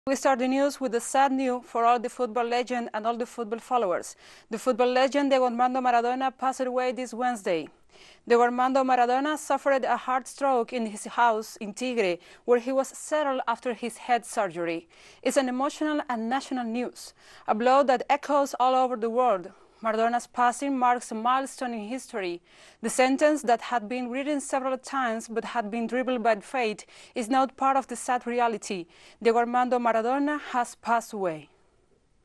Maradona passed away at age 60
Latin American soccer idol Diego Maradona Wednesday died at the age of 60 after suffering a heart attack at his home in San Andres neighborhood, Buenos Aires, Argentina. teleSUR